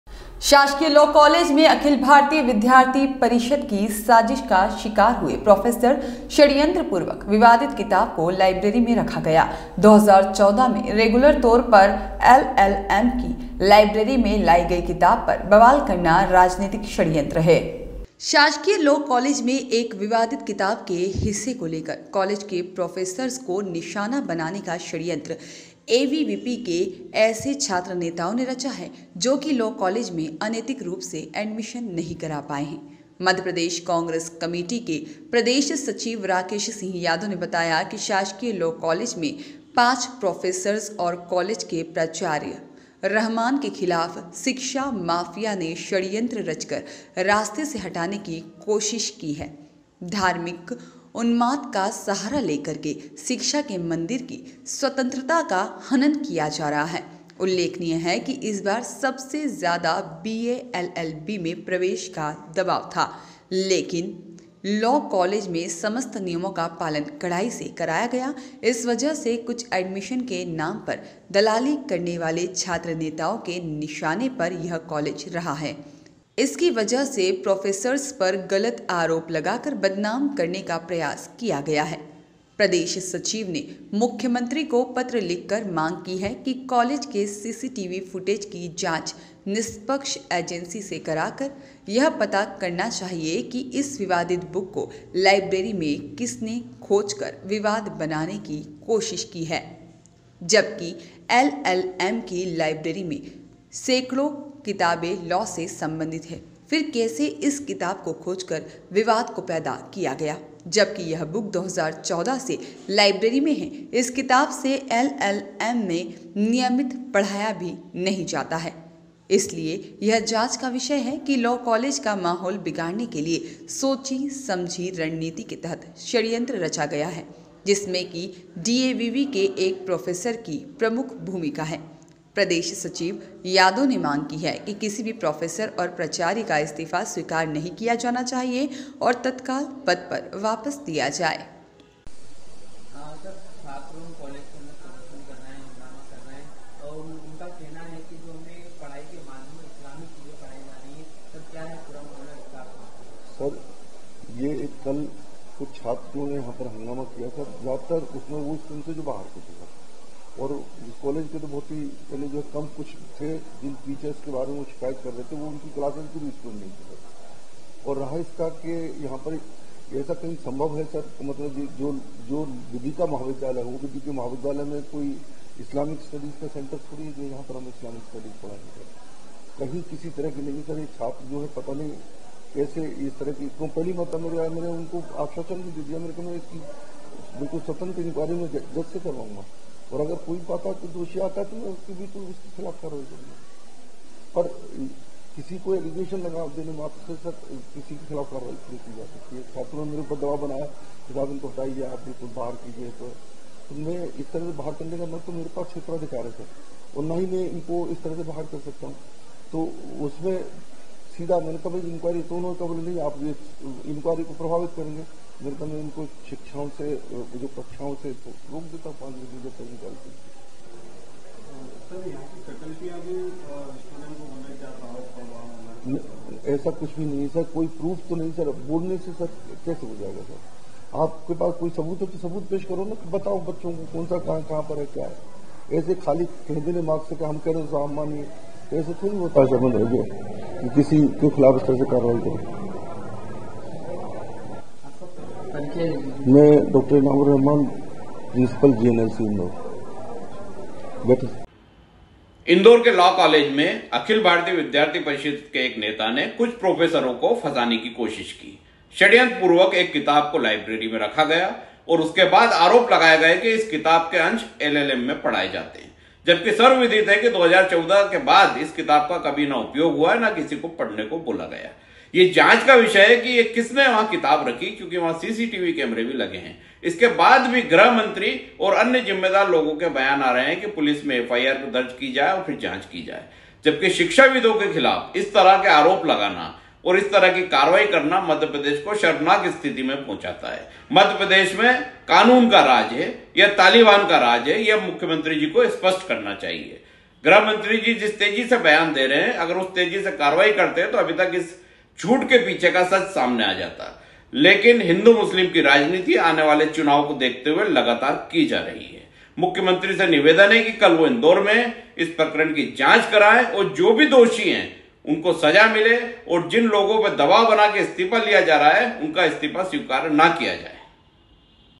लॉ कॉलेज में मुस्लिम प्रोफेसर हुए एबीवीपी की साजिश का शिकार
शासकीय लॉ कॉलेज में एबीवीपी की साज़िश का शिकार हुए प्रोफ़ेसर
षड्यंत्र पूर्वक विवादित किताब को लायब्रेरी में रखा गया”
2014 में रेग्युलर तौर पर एलएलएम की लायब्रेरी में लायी गई किताब पर बवाल करना राजनैतिक षड्यंत्र